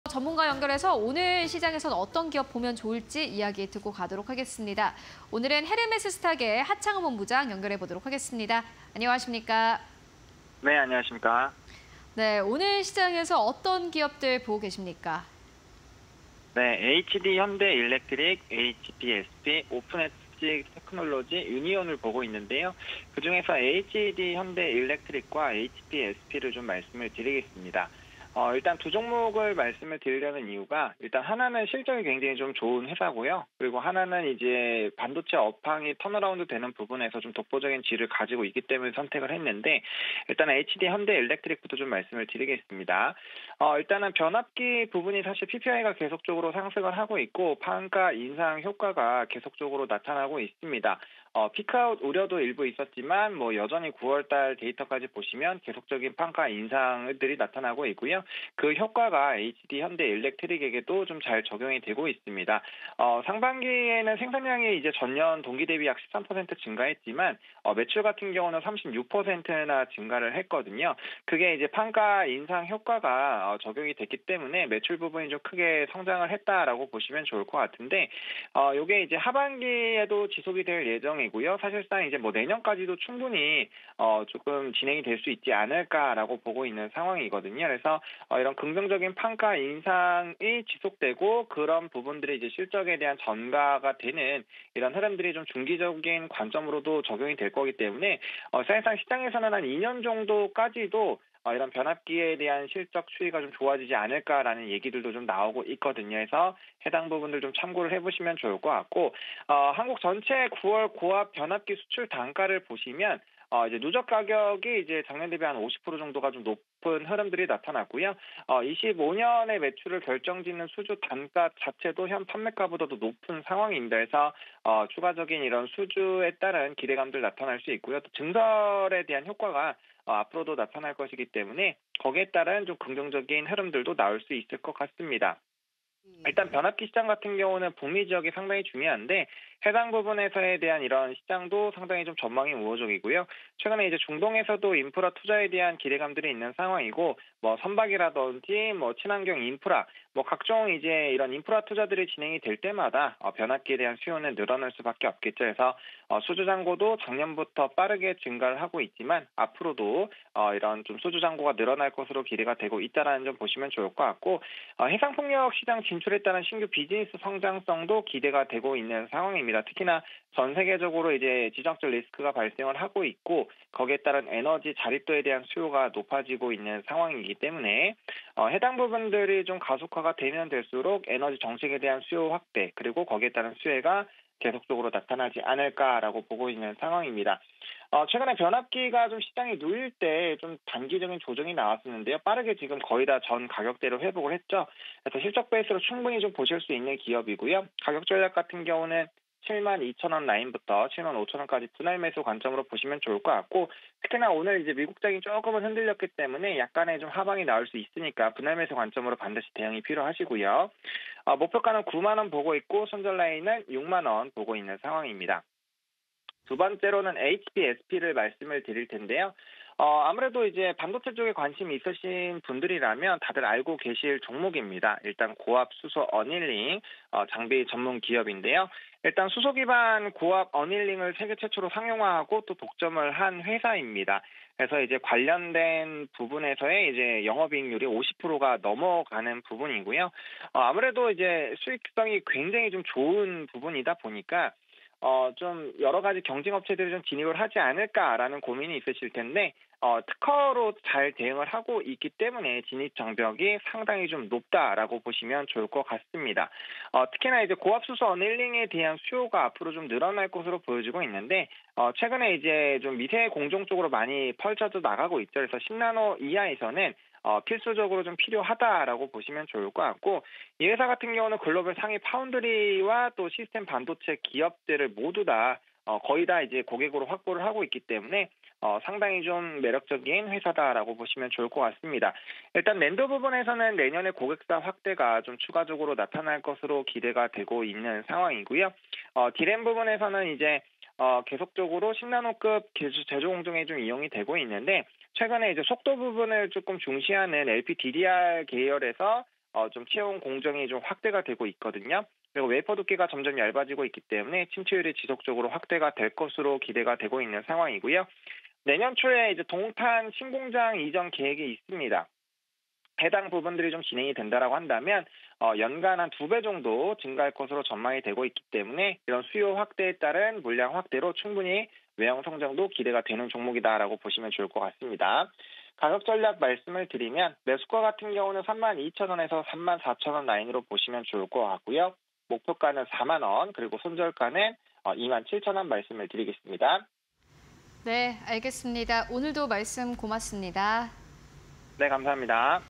HD현대일렉트릭(267260), HPSP(403870), 오픈엣지테크놀로지(394280), 유니온(000910)_하창완의 주력 종목 (20231019)
# 하창완의 주력 종목
1. HD현대일렉트릭(267260)
2. HPSP(403870)
3. 오픈엣지테크놀로지(394280)
4. 유니온(000910)

HD현대일렉트릭(267260)
- 판가 인상 따른 실적 개선 흐름
- 올해 상반기 생산량, 전년 대비 약 13% 증가
- 매출 36% 증가… 올해 매출 성장률 약 25%
- 판가 인상 효과, 하반기 지속 전망
- 9월 고압 변압기 수출 단가, 높은 수준 기록
- 수주 단가, 현재 판매가보다 높은 편
- 공장 증설 효과 지속… 추가 수혜 기대
- 북미·중동 선박 시장 전망 '긍정적'
- 변압기 수주 잔고 증가세
- 해상 풍력 시장 진출…성장성 가시화
- 중동 지역 신재생에너지 비중 확대

HPSP(403870)
- 세계 최초 고압 수소 어닐링 장비 개발
- 독보적 기술력으로 시장 독점
- 글로벌 상위 파운드리 기업 등 고객사 확보
- 2024년 낸드 부문서 고객사 확대 예상
- D램 부문 수요 확대 기대
- 고압 산화막 장비 신규 공급
- 산화 공정 신규 솔루션 관련 고객사 관심↑
- 신공장 이전 후 생산력 2배 이상 증가 전망